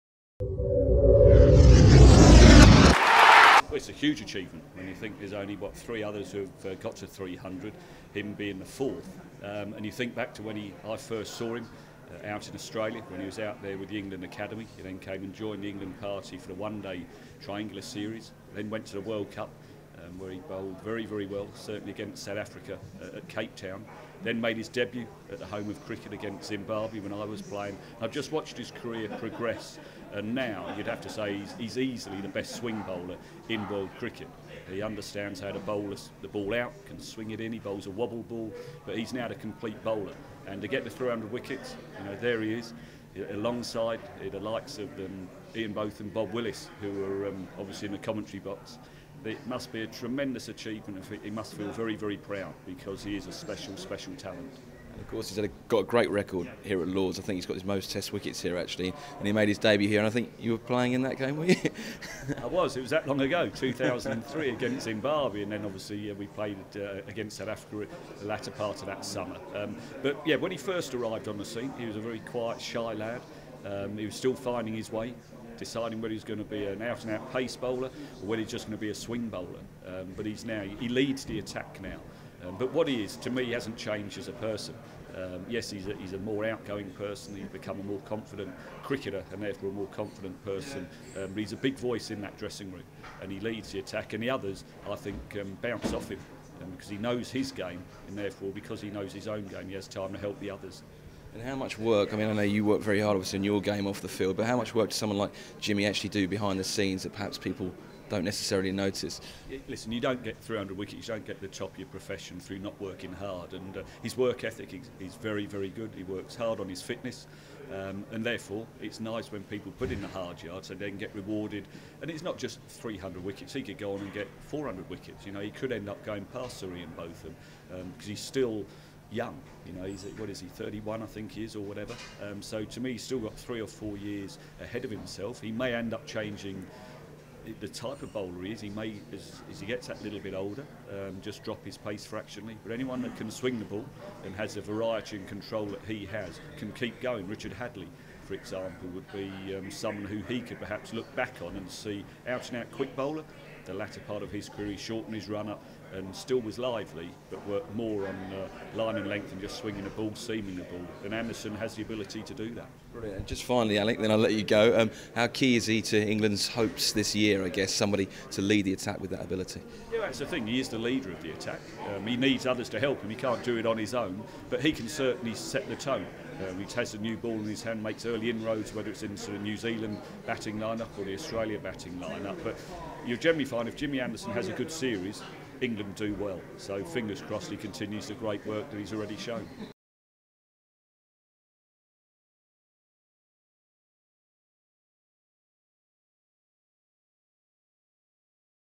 Exclusive: Alec Stewart hails Anderson's 300 wickets
Former England captain and legend Alec Stewart spoke to ECB Online today and was full of praise for James Anderson, who has just joined the exclusive '300 club.' Anderson took his 300th wicket at Lord's against New Zealand and took his place in history amongst the greatest bowlers to have played the game.